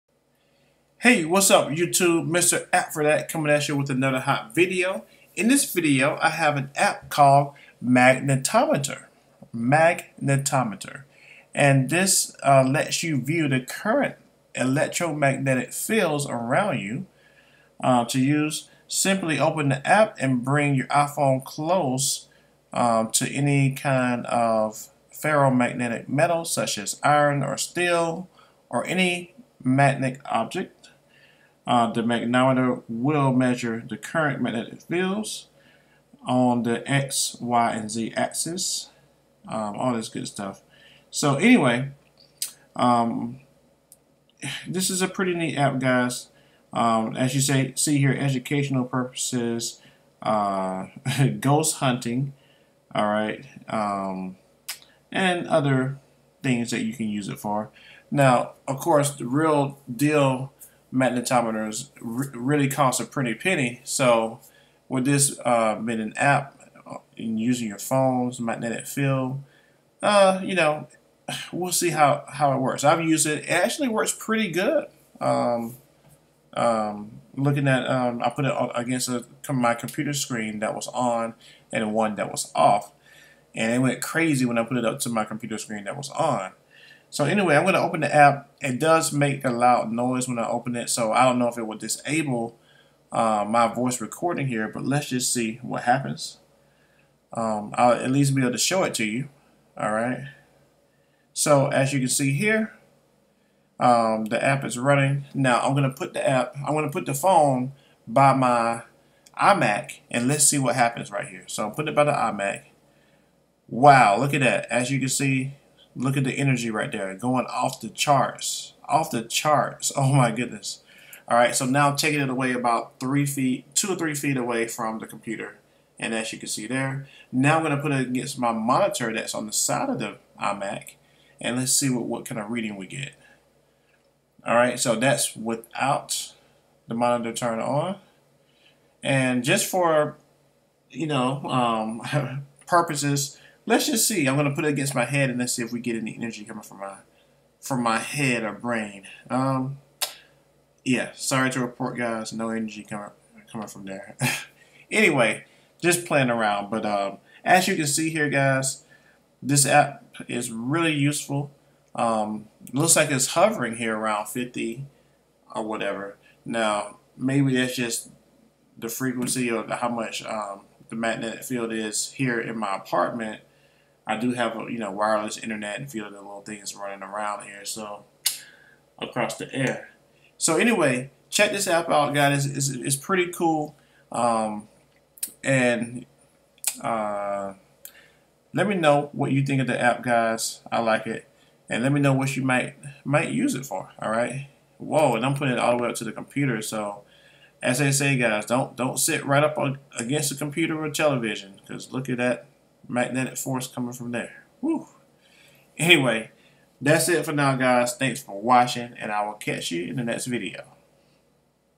Magnetometer
Magnetometer lets you view current electromagnetic fields around you! To use, simply open Magnetometer and bring your iPhone close to any ferromagnetic metal such as iron or steel, or any other magnetic object. Magnetometer will measure the current magnetic fields on the X, Y, and Z axis in micro-teslas. In addition Magnetometer displays a magnitude reading of (X^2+Y^2+Z^2).

Here are some practical ways of using Magnetometer:

Metal detector: Imagine yourself buying some gold coins. By using Magnetometer you could tell if they were real or fake by holding the coin up to your iPhone. If the magnitude goes up dramatically, you can be sure that the coin is fake.

Educational purposes: You're sitting in science class and the teacher starts talking about electromagnetic fields. Impress your teacher by showing them your fully portable Magnetometer on your phone! Use Magnetometer in various science experiments while your fellow classmates watch you in disbelief.

Ghost Hunting: Use Magnetometer like a real EMF detector! A very common theory is that ghosts disrupt magnetic fields, thus a person can find ghosts by viewing disturbances in magnetic fields. Whether you're a professional or an amateur, Magnetometer is the app for you!

How exactly does it work?
Since the iPhone 3GS/iPhone 4 includes a compass, Magnetometer can measure the interference created by a ferromagnetic metal (or paranormal activity). Magnetometer is NOT a true magnetometer and shouldn't be used for anything but entertainment purposes.